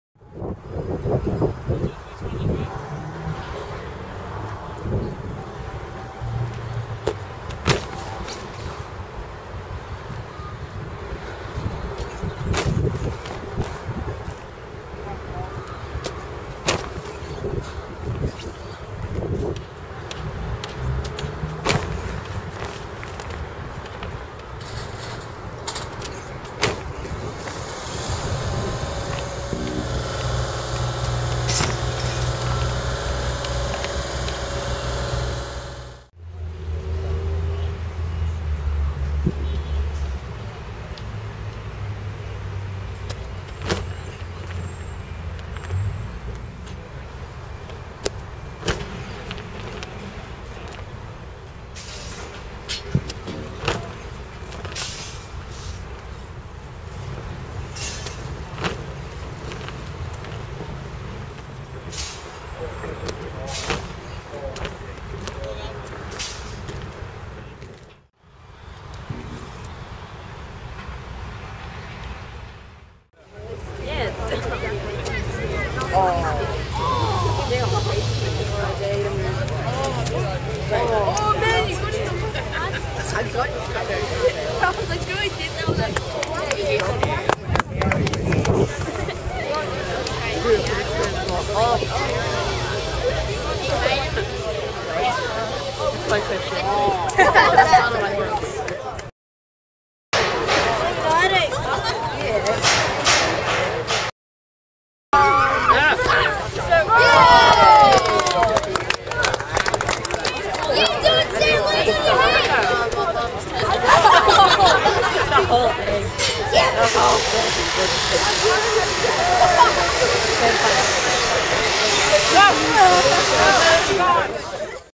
Front Flips Training 1st March 2013
I had an audience today; a local primary school was having an athletics day at the track.
It was fun to perform, amazingly kids will watch a guy practise the same technique for 20 minutes straight.